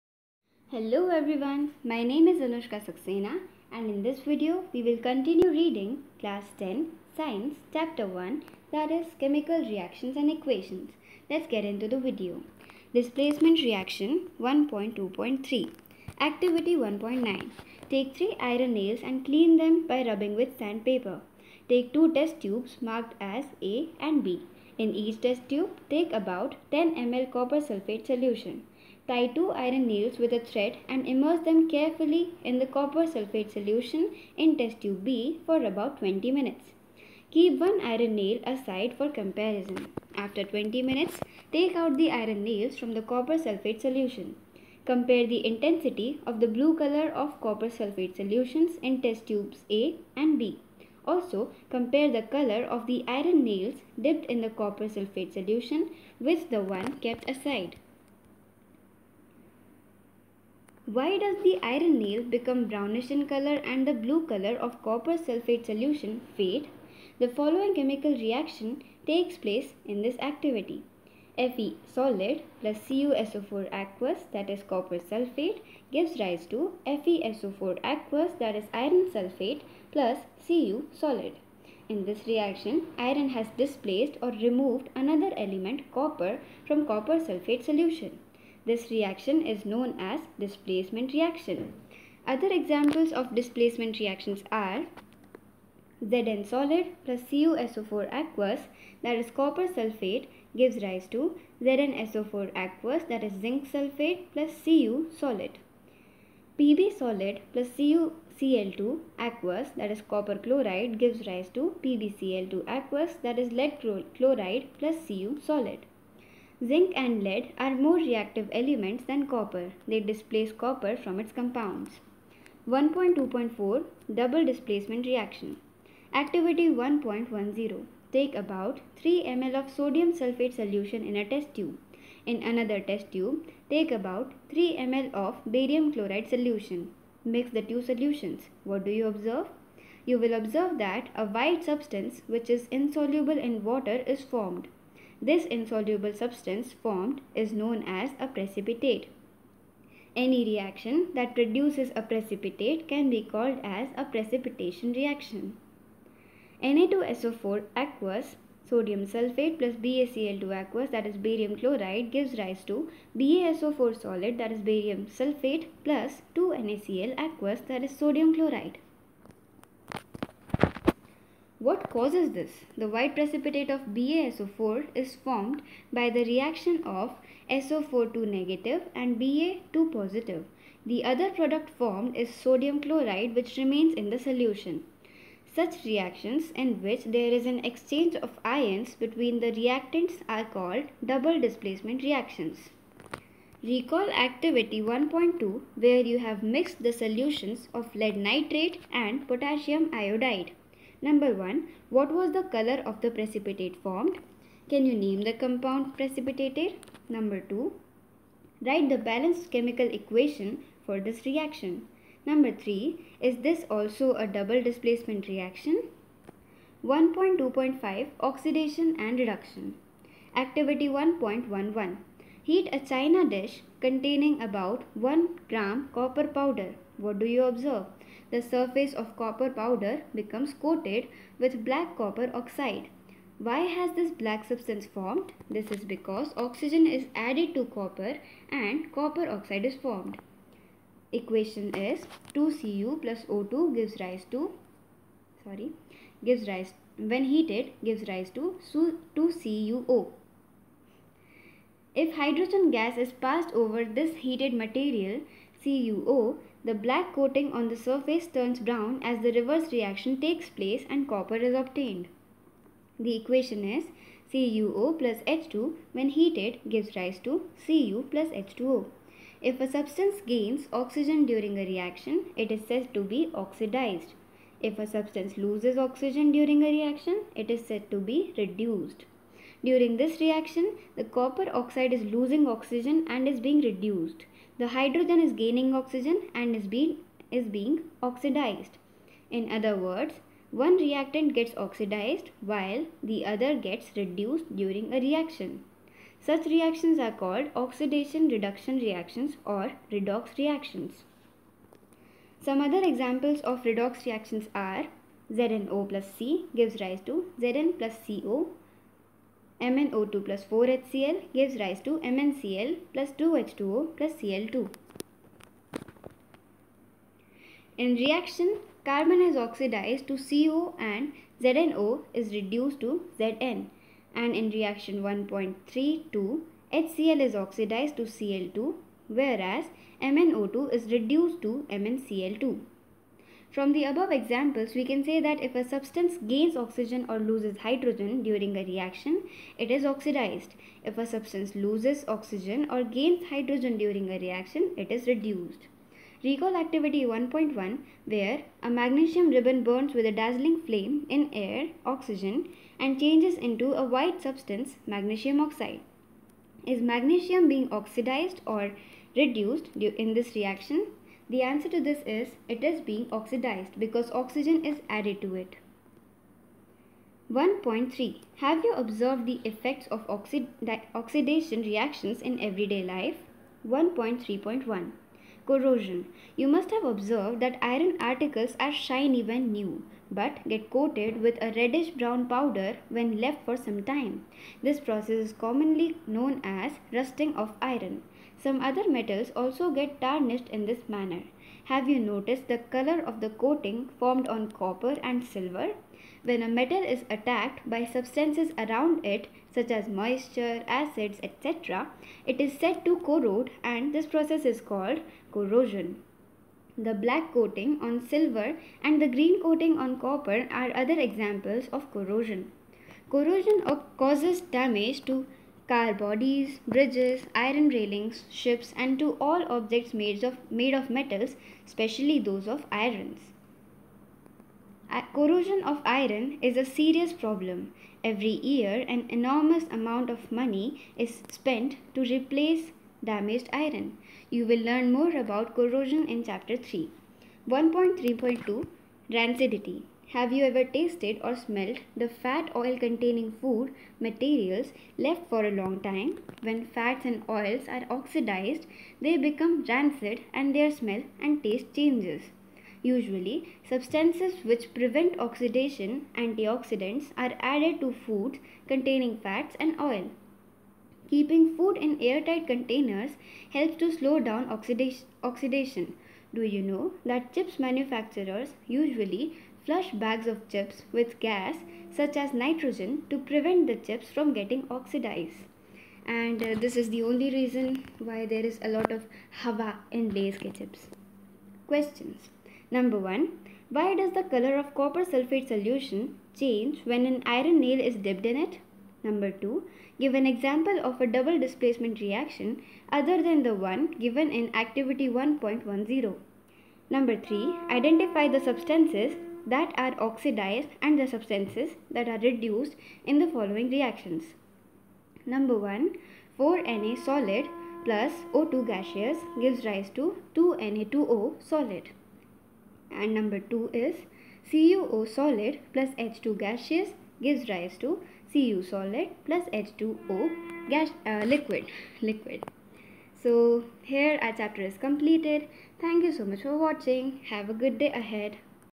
(Reading only) Chemical Reactions And Equations | Part 3/3 | Class 10 | Science | Anushka Saxena
PART 1= = "Intro" to "1.1 Chemical equations"
PART 2 = = "1.2 Types of chemical reactions" to "1.2.2 Decomposition reaction"
PART 3 = = " DISPLACEMENT REACTIONS" to "RANCIDITY (END OF CHAPTER)"
Sorry for bad audio quality, if it is really bad then please let me know, I'll re-upload the video.
Class 10
THANKYOUUU
XOXO